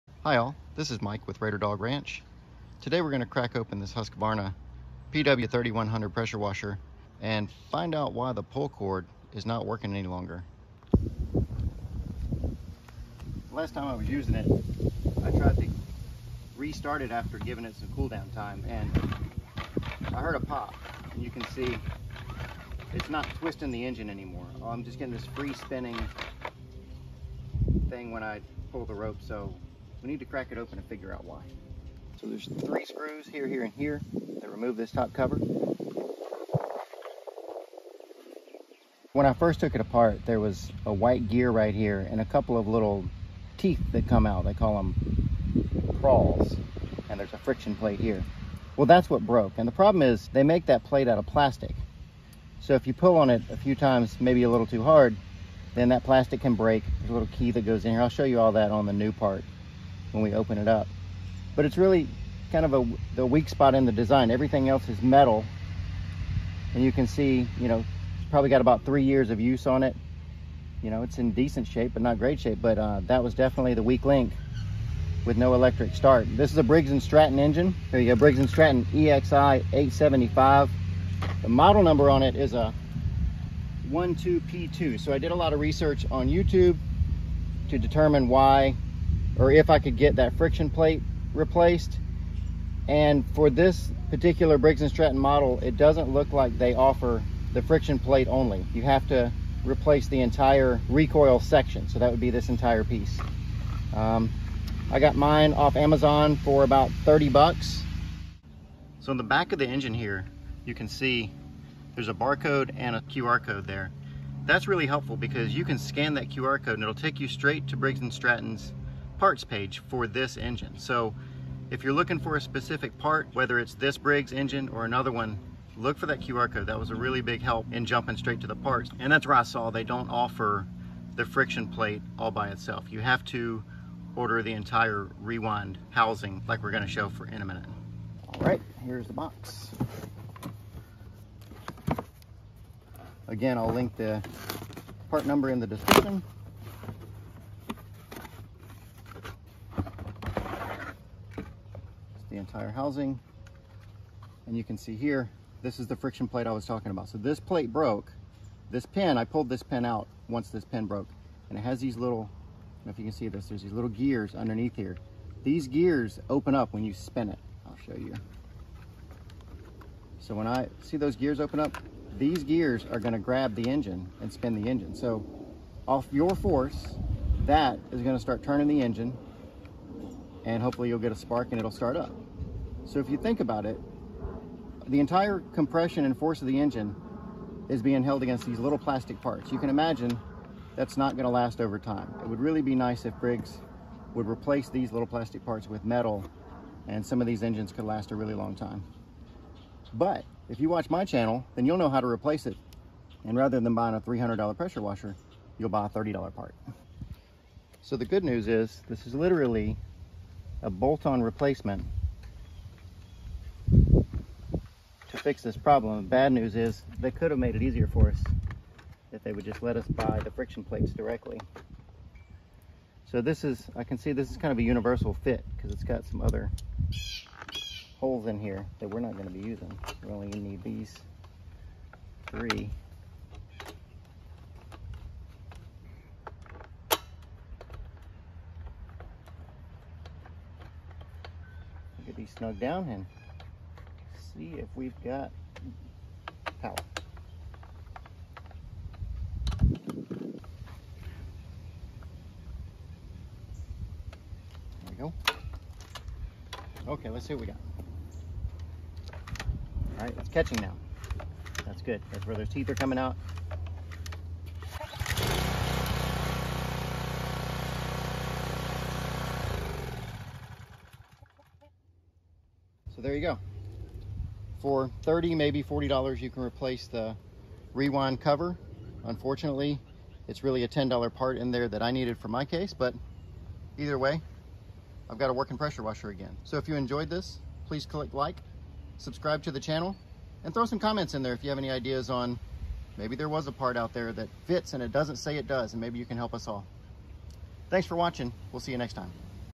Husqvarna pressure washer with Briggs and Stratton Engine Won't Start - Broken Friction Plate
How to diagnose and replace a broken friction plate in a Briggs and Stratton 124P2-0001-F1 engine. The pawl friction plate broke when trying to start my Husqvarna pressure washer. This engine is used on a lot of different platforms like lawn mowers, pressure washers, etc.

Briggs and Stratton 594062 Rewind Starter